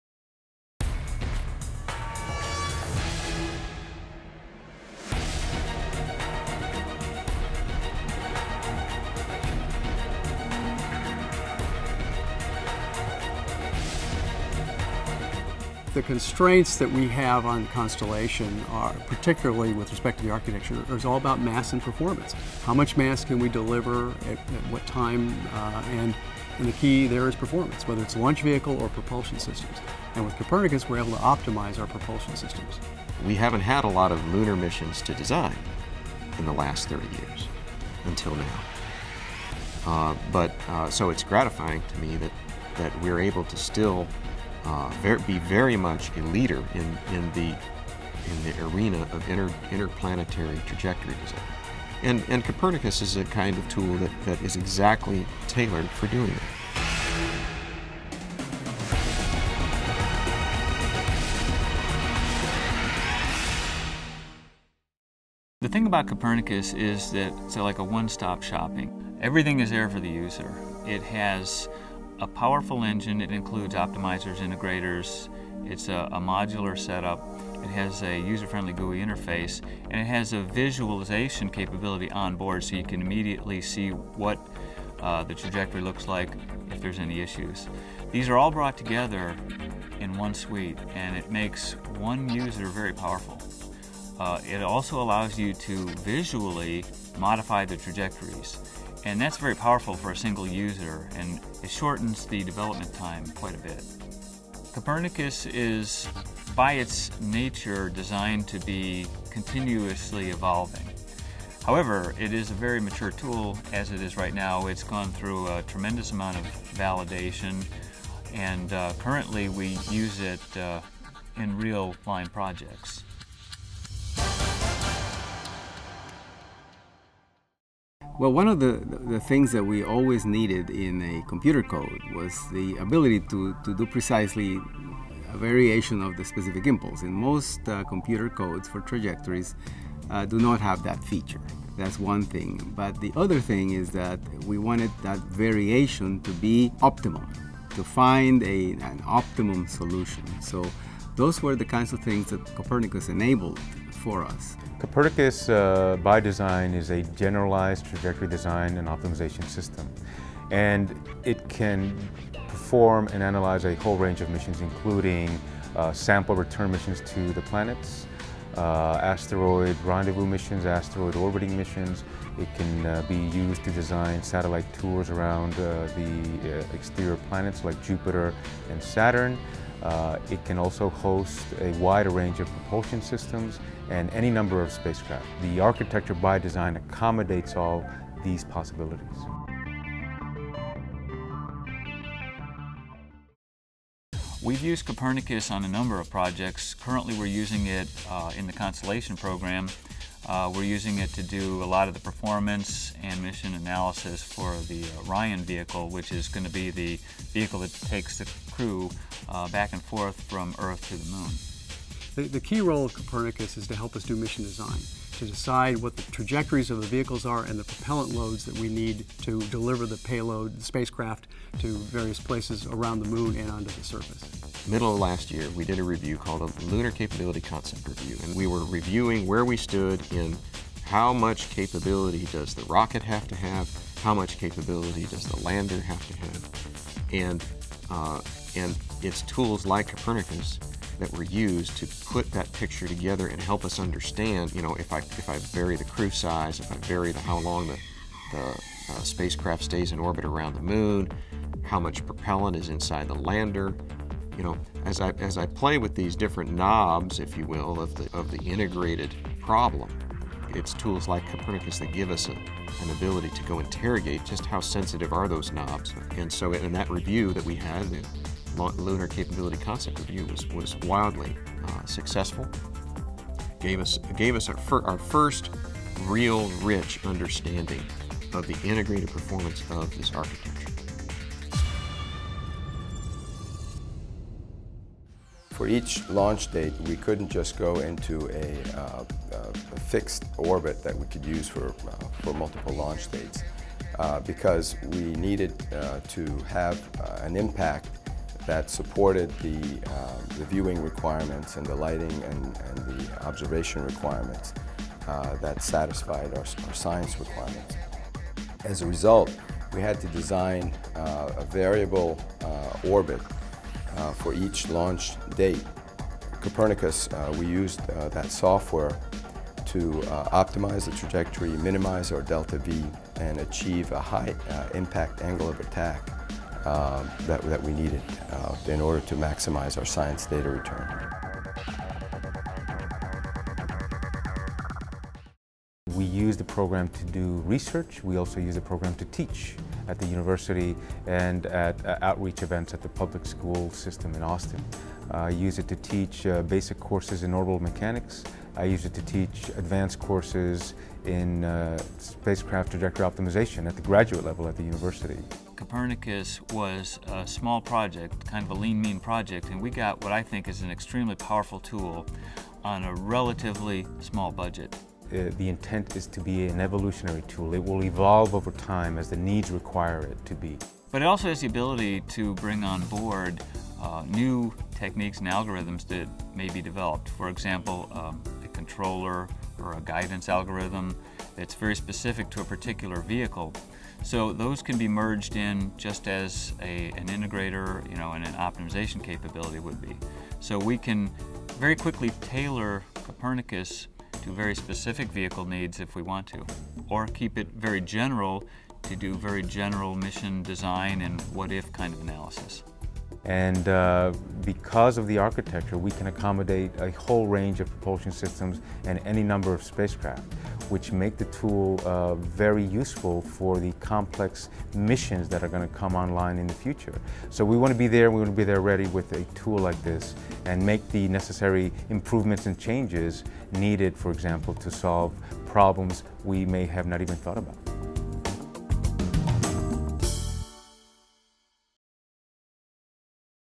Copernicus Trajectory Design and Optimization System
The Copernicus Trajectory Design and Optimization System represents a new and comprehensive approach to performing mission design, trajectory analysis and optimization.

Copernicus brings together the state-of-the-art in trajectory optimization techniques, visualization, an easy to use GUI, a library of key algorithms, and a distributed (batch) processing capability into an integrated package.

It's a "one stop shopping" tool for interplanetary mission and trajectory design optimization and analysis.